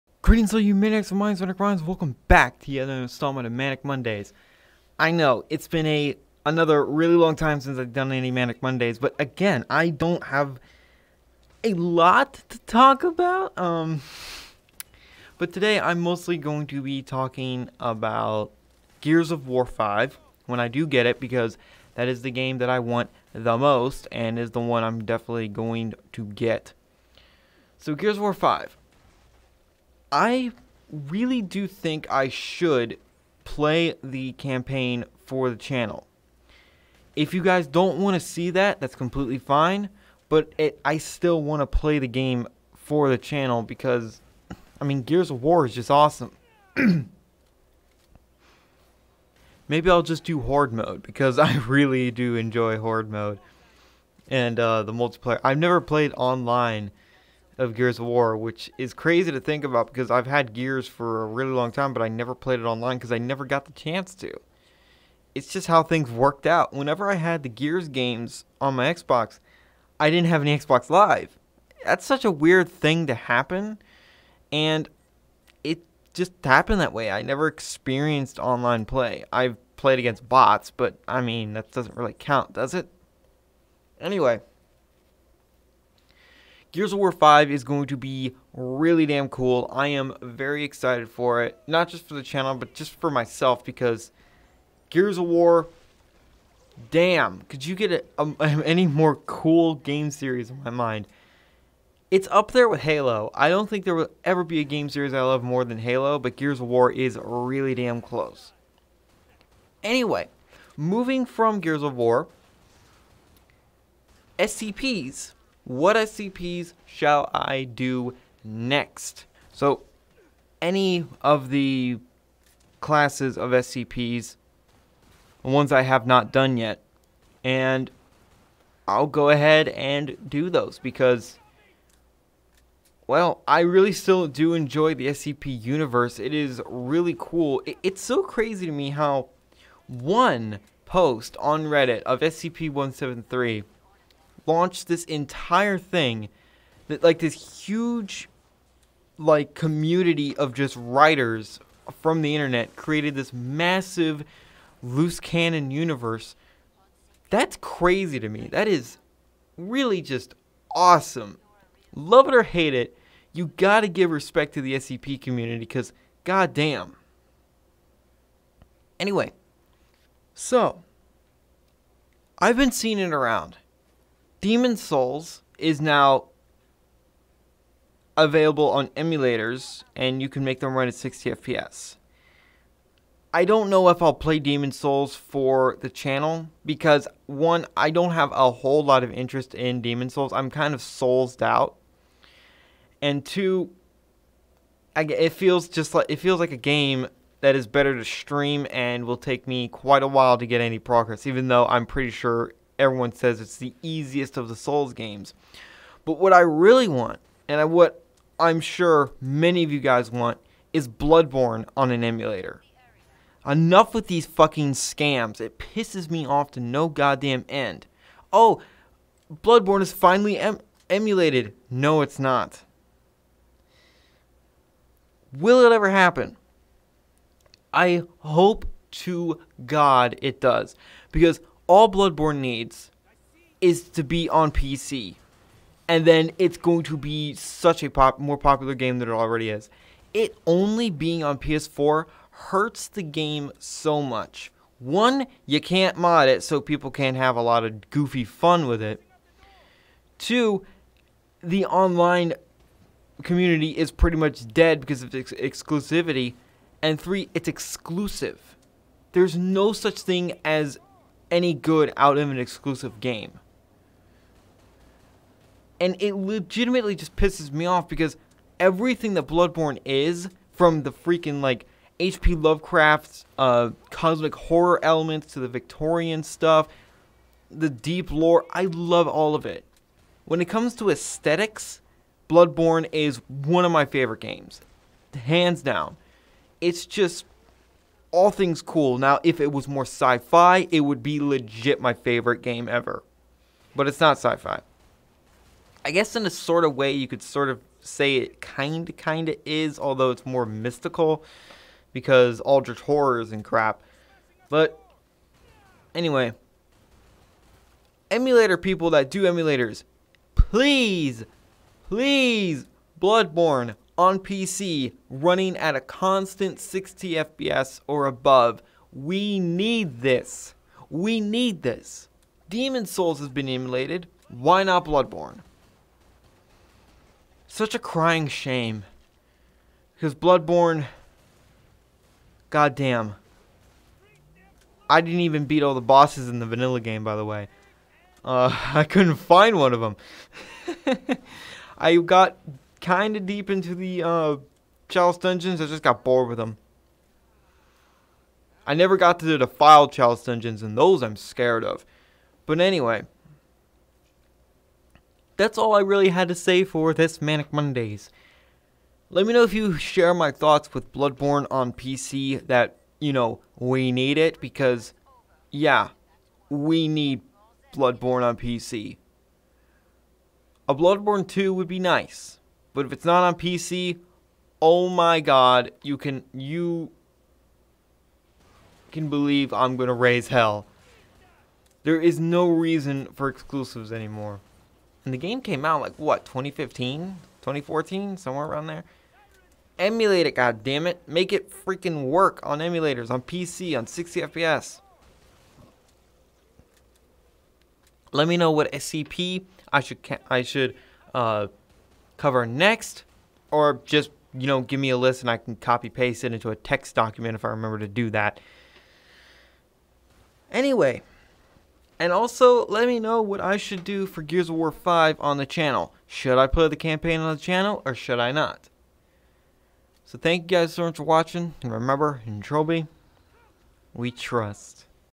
Gears Of War 5, More SCPs And Other Stuff | Manic Mondays
What shall I do for Gears Of War 5 on the channel? What SCP's to talk about next?